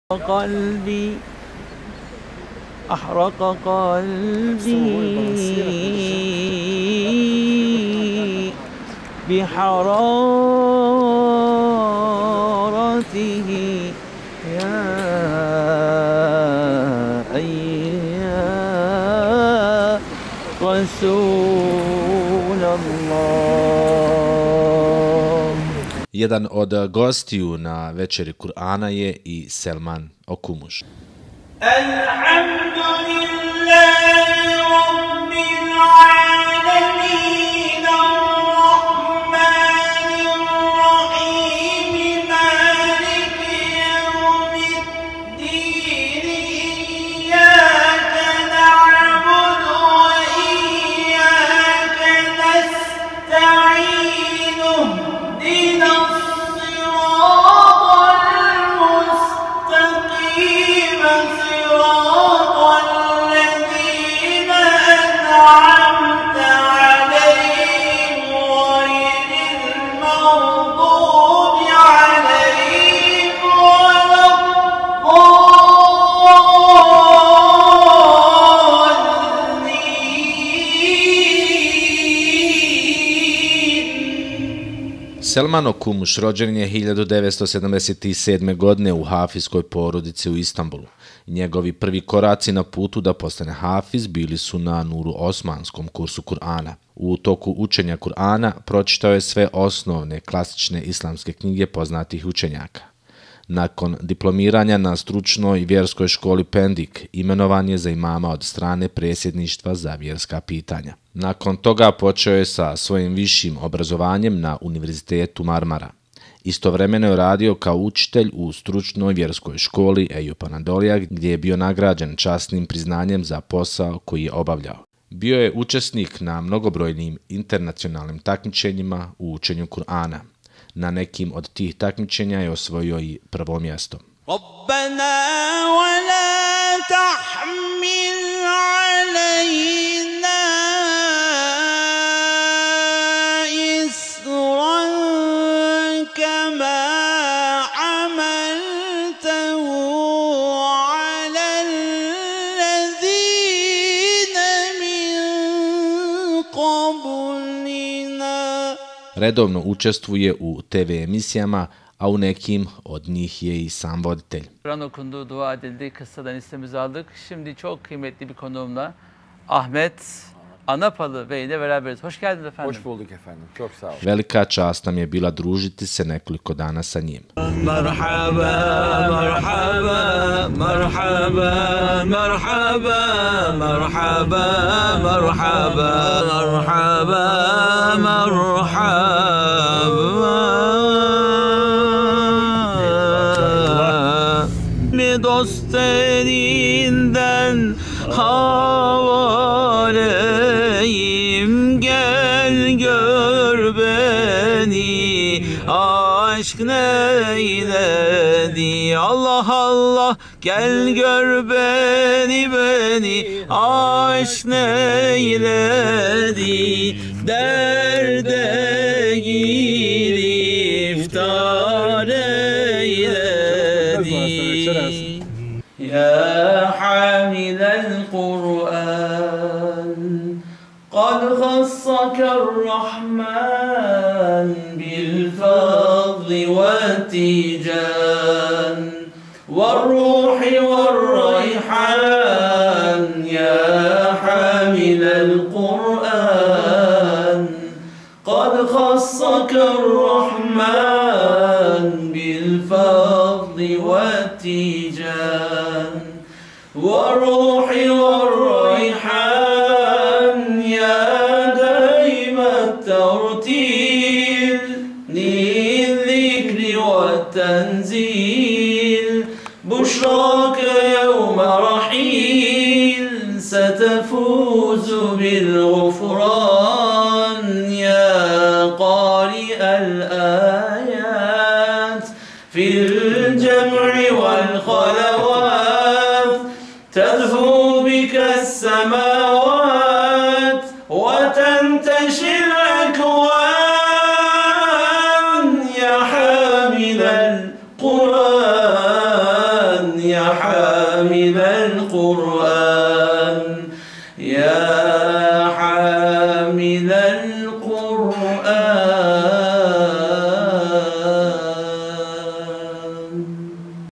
Hafız Selman Okumuş Bosna Hersek Televizyonu Belgeseli
Dünya ezbere Kur'an Okuma birincisi Hafız Selman Okumuş yaşamından kesitler, Bosna Hersek MTV Igman kanalı tarafından belgesele alınıyor.